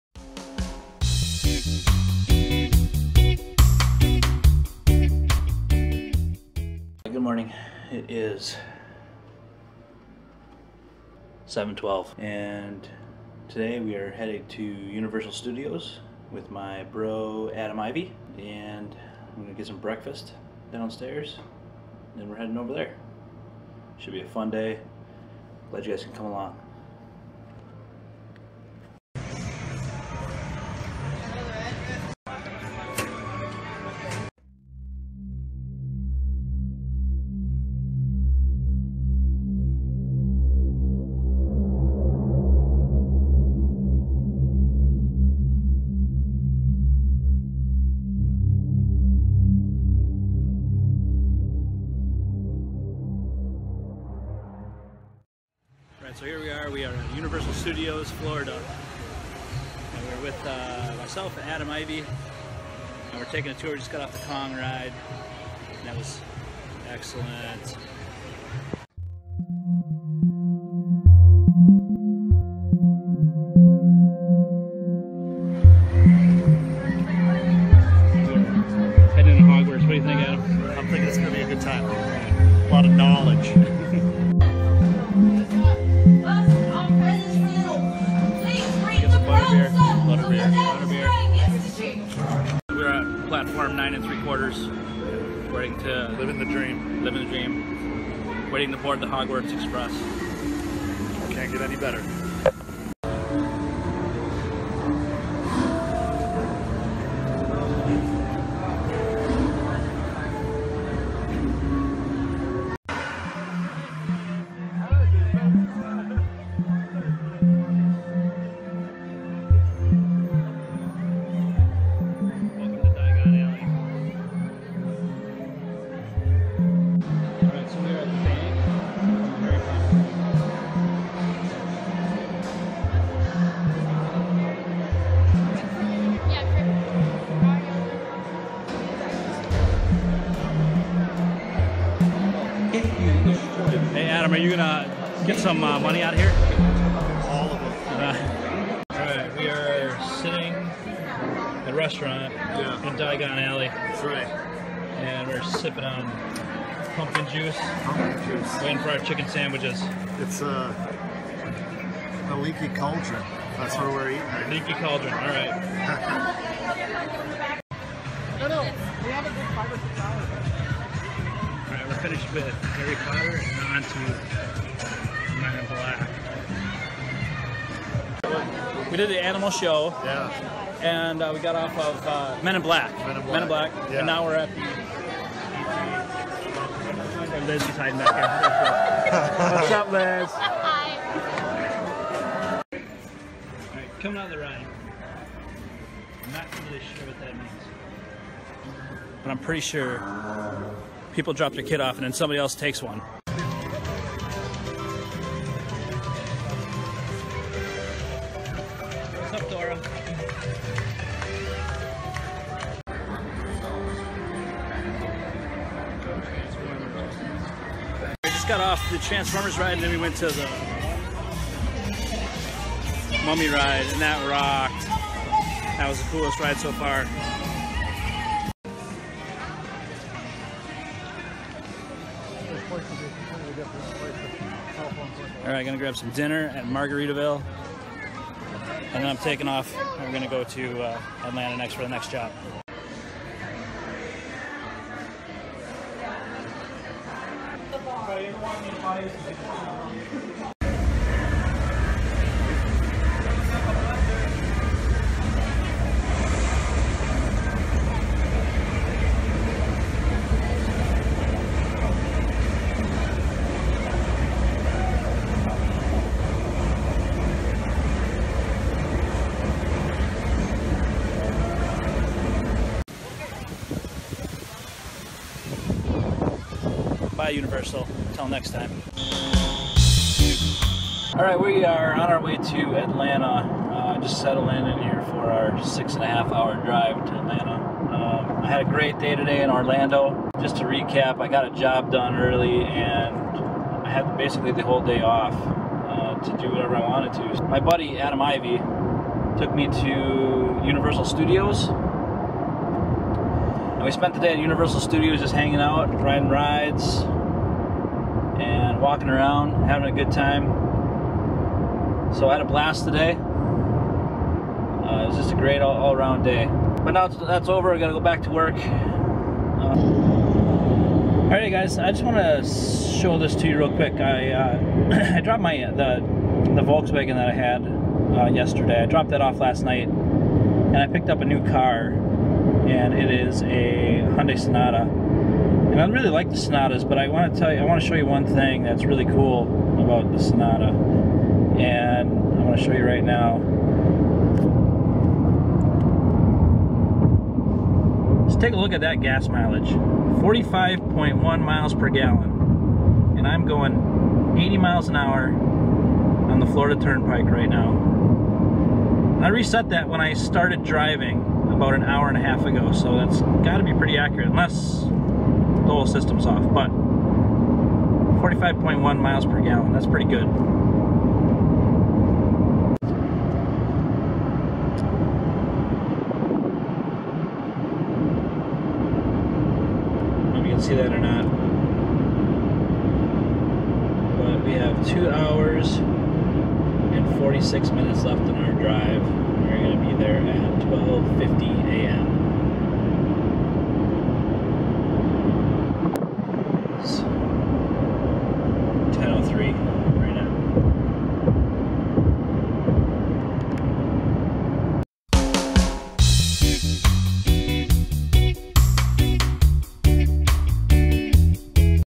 Hanging with Adam Ivy @ Universal Studios! VLOG 3
Travel Vlog 3 July 19th 2016 Orlando, FL - Lawrenceville, GA